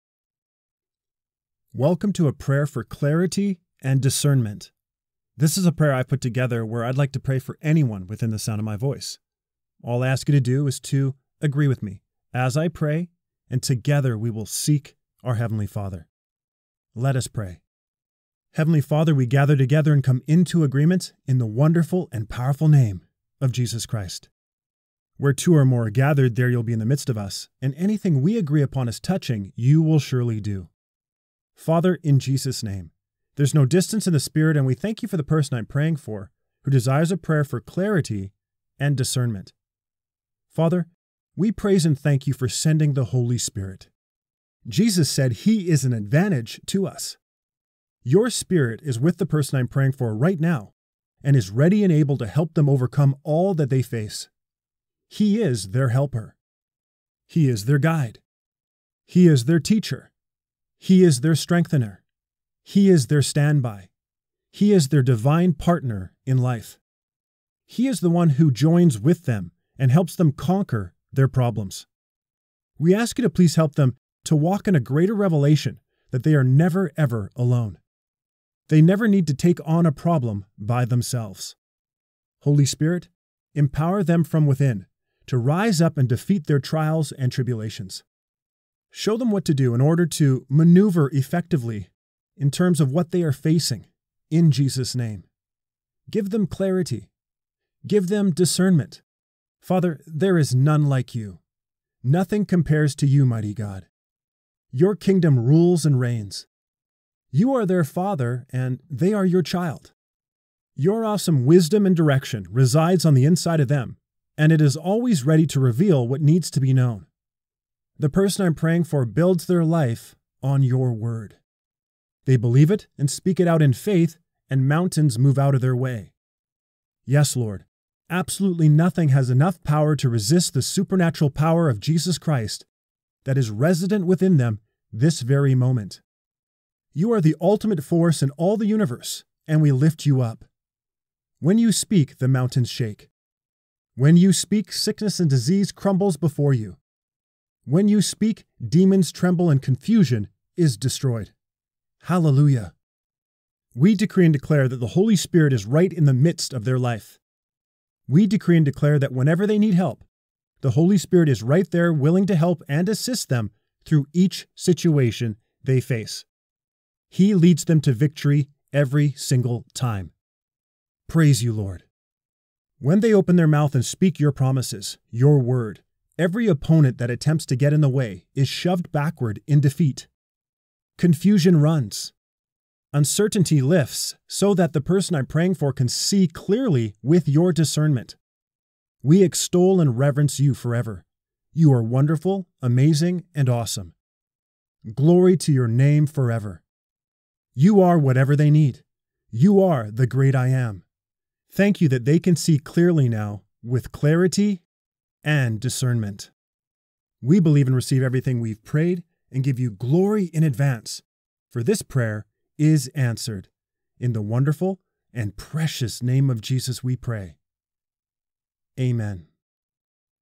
God, Guide Me | A Blessed Daily Morning Prayer For Discernment and Wisdom (PODCAST)
This is simply a podcast I’ve put together where I would like to pray for anyone within the sound of my voice.

You can adjust the speed of the audio in the playback settings.

All I ask you to do is to agree with me as I pray and we seek our Heavenly Father above all else.

Please continue to meditate on this blessed daily effective prayer for yourself.

Speak it daily or listen to this video over and over again and allow the Word of God concerning this daily morning prayer to reach deep into your spirit.

Daily effective prayer changes things.

Daniel / Daily Effective Prayer